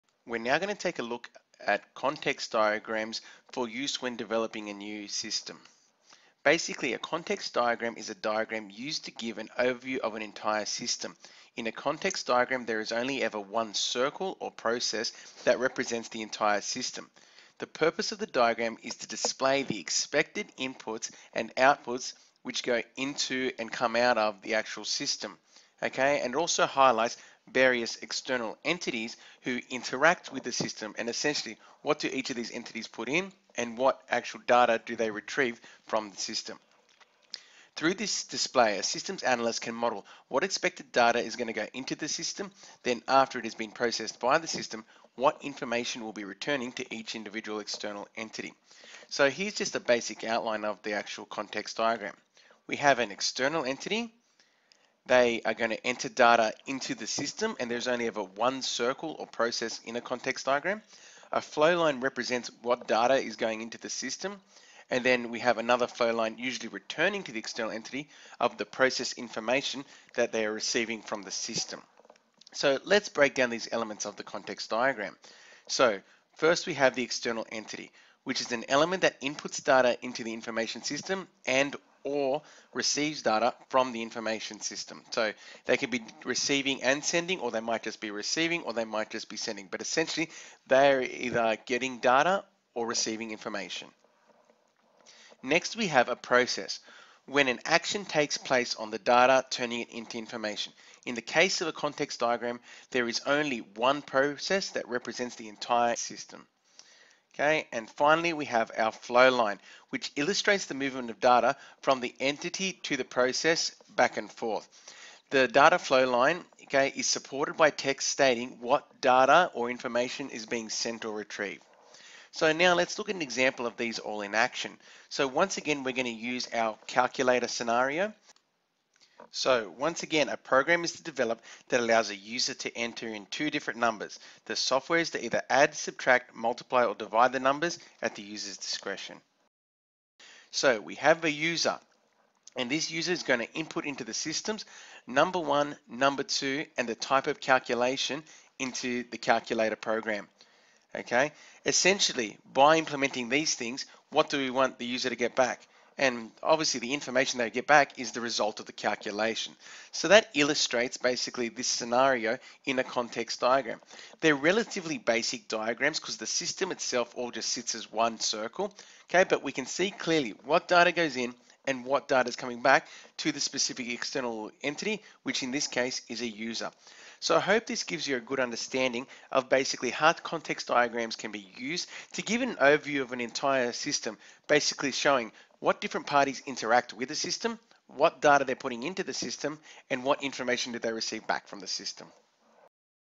Context Diagrams Overview
A diagram used to give an overview of an entire system. In a context diagram there is only one circle / process that represents the entire system. The purpose of this diagram is to display the expecting inputs and outputs from the system to and from various external entities. Through this display a system analyst can model what expected data is going to go into the system, then after it has been processed by the system, what information will be returning to the external entities.